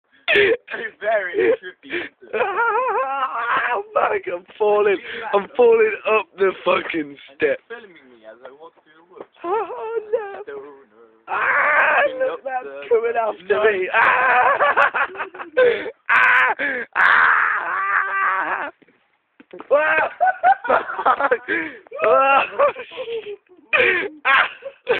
We B Stoners Episode 34
Matt falls up the steps and something comes after him. Mike likes to think he's flying :P